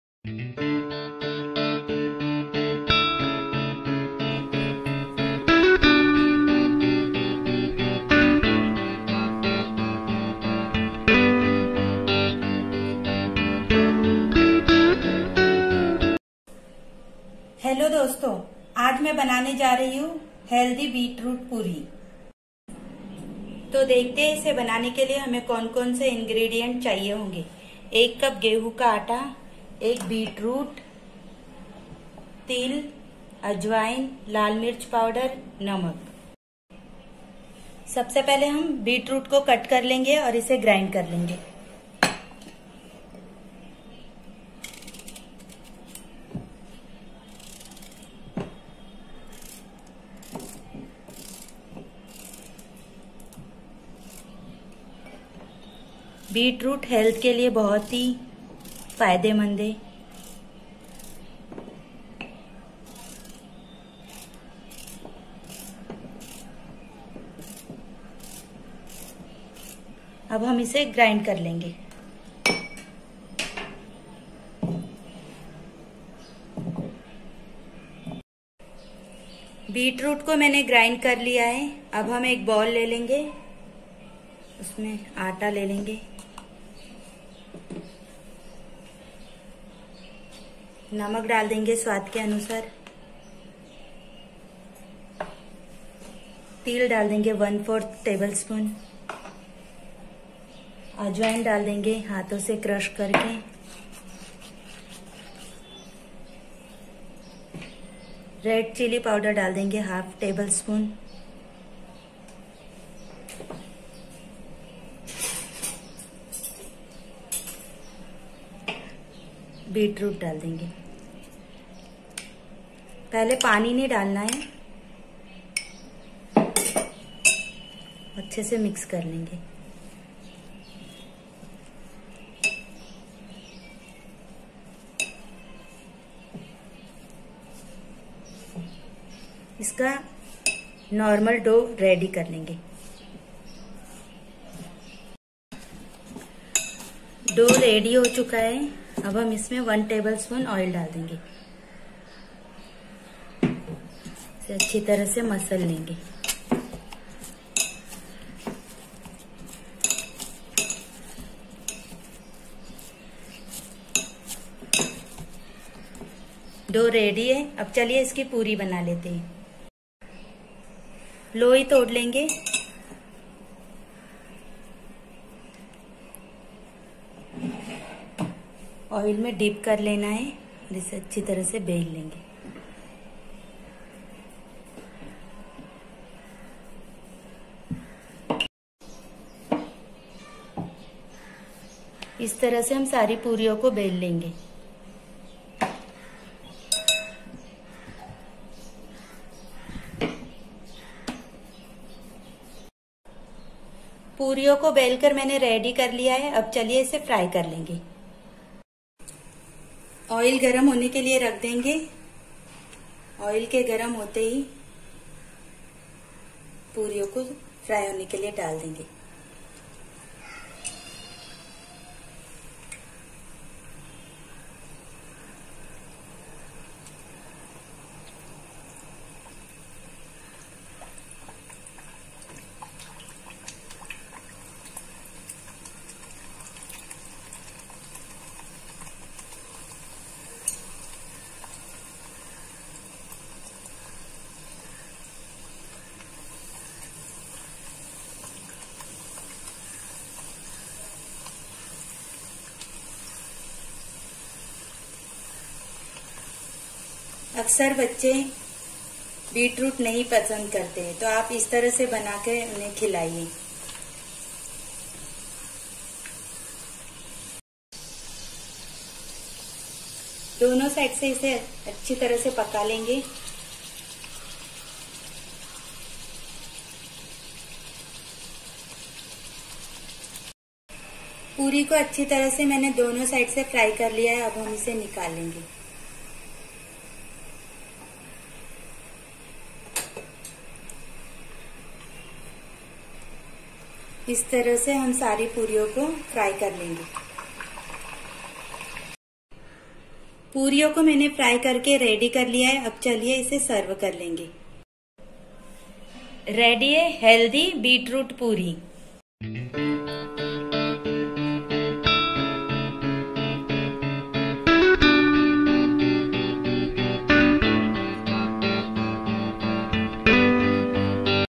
Beetroot puri | Healthy puri recipe | Lunch box recipe
Ingredients:
1. Beetroot
2. Wheat flour
3. Salt
4. Red Chilli powder
5. Ajwain
6. Sesame seeds

Procedure:
Take a bowl add one cup of wheat flour, salt, red chilli powder, ajwain, sesame seed, grinded beetroot and mix it properly. Make a soft dough of it.
Make puri of that dough. Heat a pan add oil and fry all the puris as shown in the video.
Healthy beetroot puri is ready to serve:)